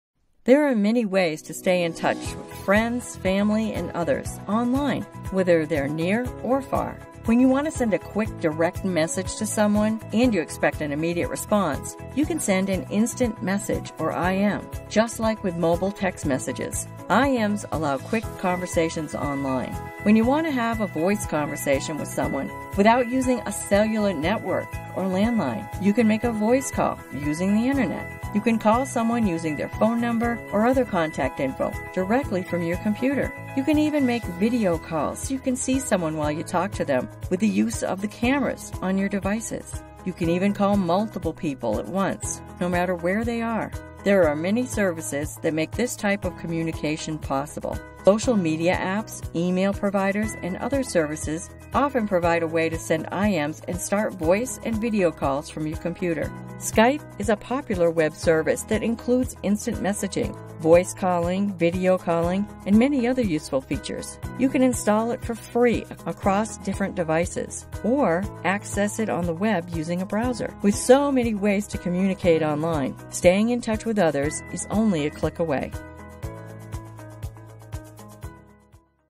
06 | Intro to IM. voice & video calling | Communicate Online | Microsoft Digital Literacy.
There are other useful tools. Instant messaging (IM) is a way to chat with someone in real time, as if you're in a live conversation. Online voice calling (VOIP) allows you to make calls to other online users or phone numbers, all without using a landline or cellular network. With video calling, you can use a web camera to see the person you're talking to on the screen.
Watch the following video to learn more about IM, voice calling, and video calling.